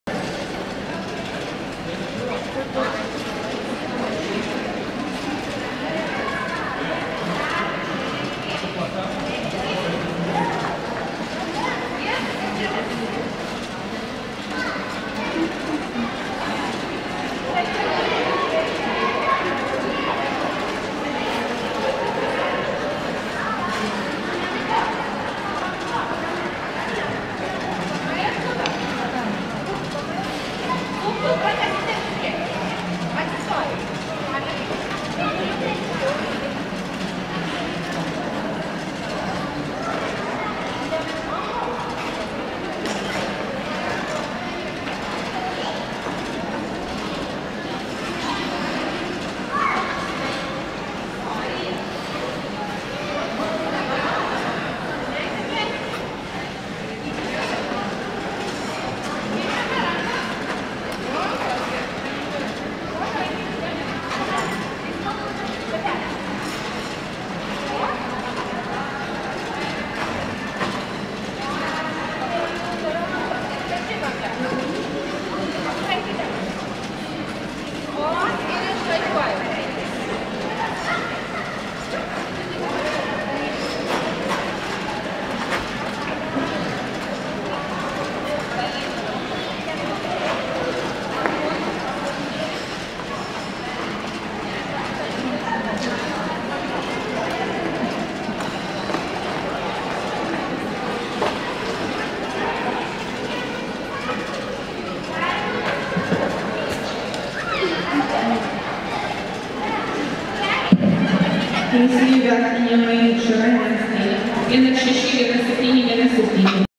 Nastia S. | Elementai | Chicks D 2010/2011 | Vilnius 2017-12-01
"Sidabrinė pačiūža" Dailiojo čiožimo elementų ir programų varžybos
Elementai
Chicks D 2010/2011